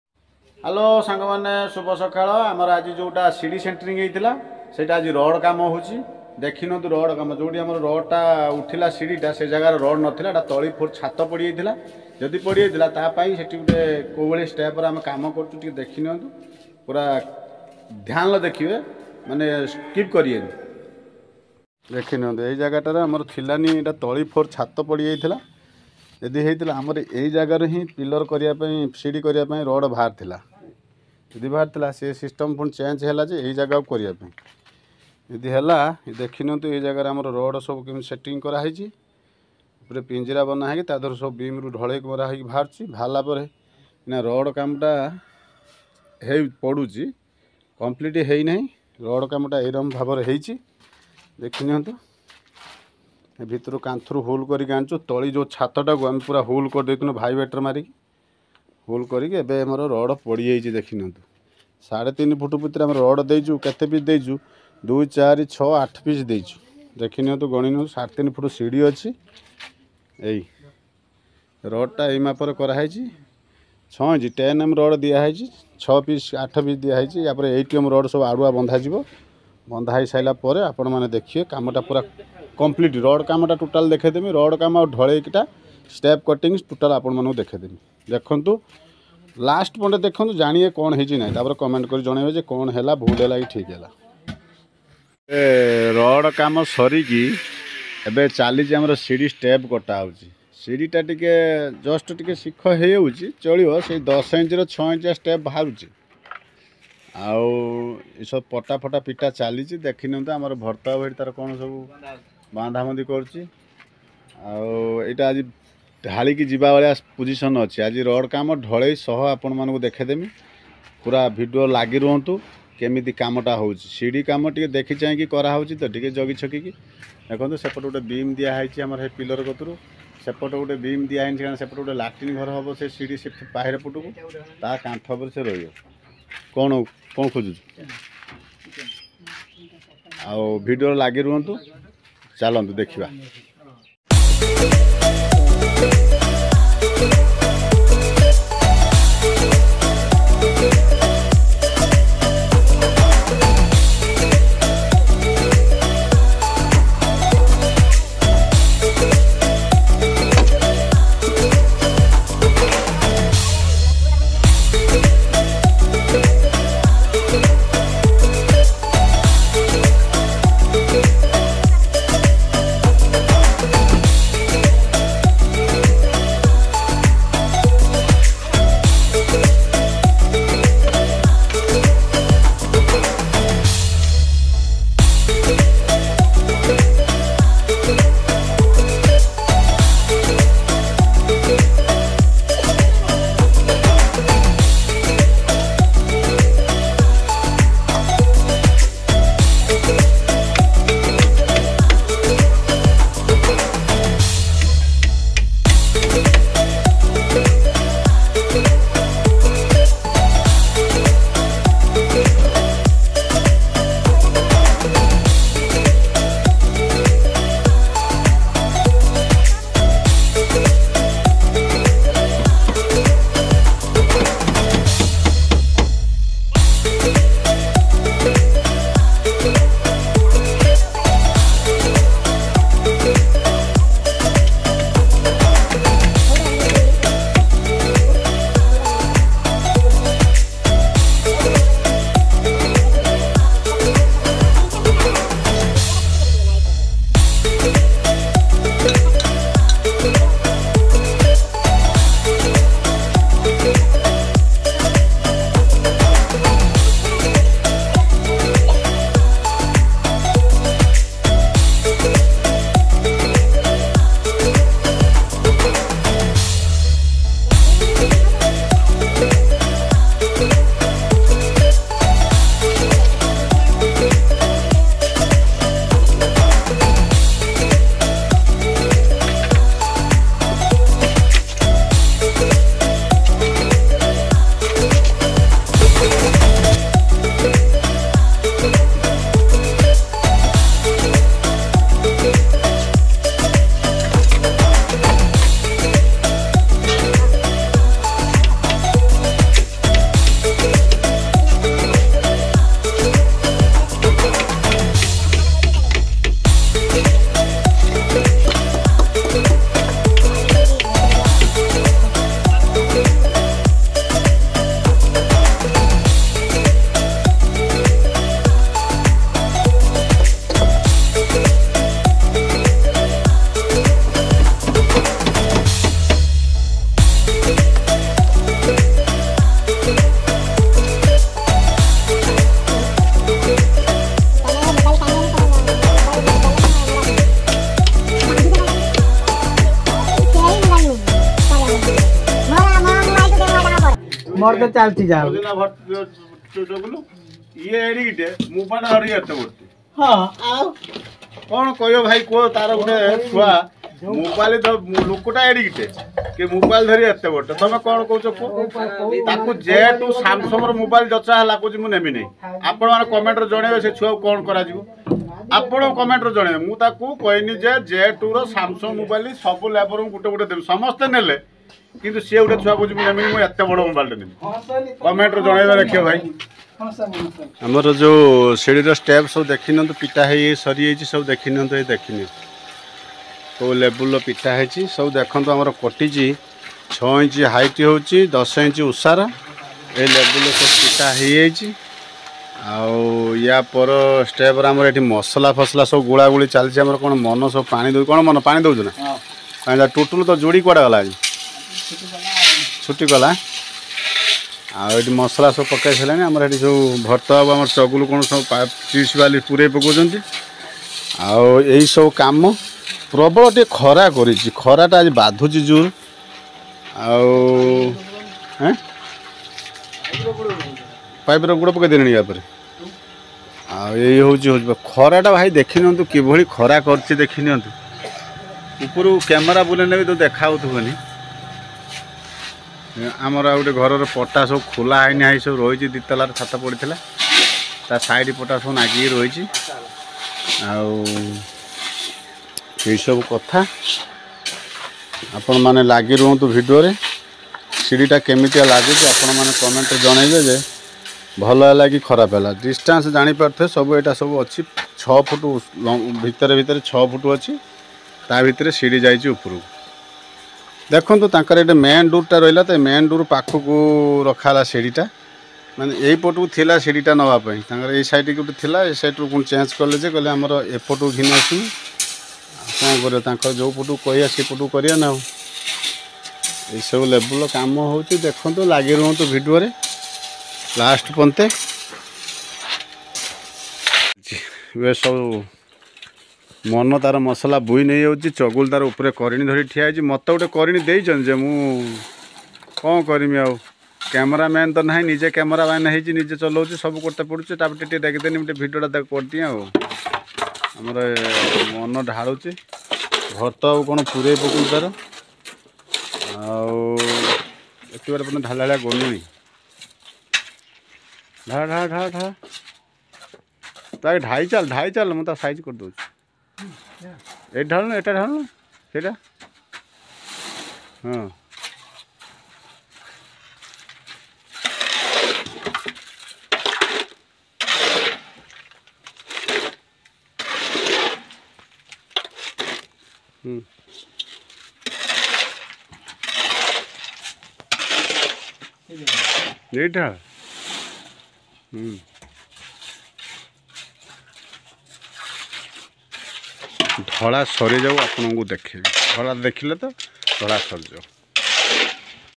How to Calculate Rise and Tread of Staircase/Staircase staps cating/ଆସନ୍ତୁ ସିଖିବା ସିଡିର ଷ୍ଟେପ cating
ଏହି ଭିଡିଓରେ ଆପଣମାନେ ଦେଖିବାକୁ ପାଇବେ କି ଘରର ସିଡ଼ିର ଷ୍ଟେପ୍ କିପରି କଟିଙ୍ଗ୍ କରାଯାଏ.